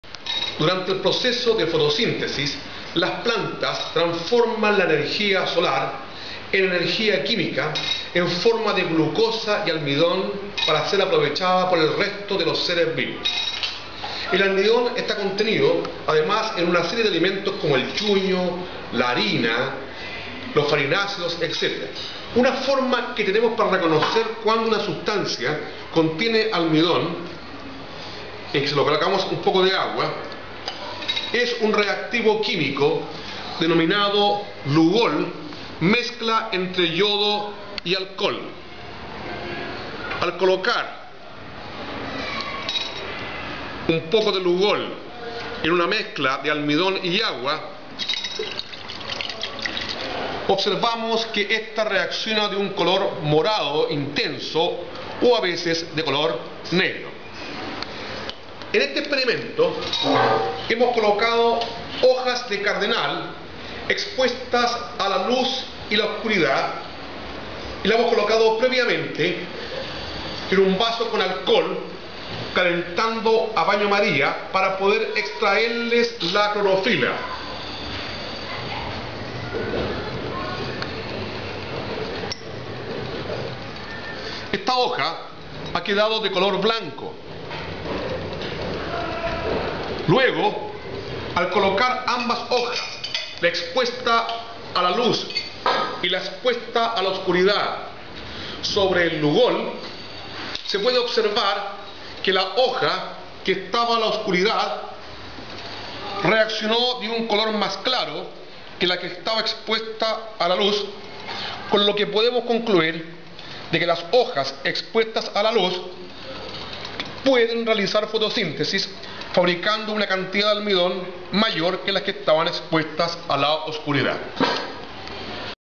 fotosintesis
Se observa como al extraer la clorofila a hojas expuestas a la oscuridad y luz , estás últimas al ponerlas en lugol, reaccionan de un color más oscuro detectando un  mayor cantidad de almidón.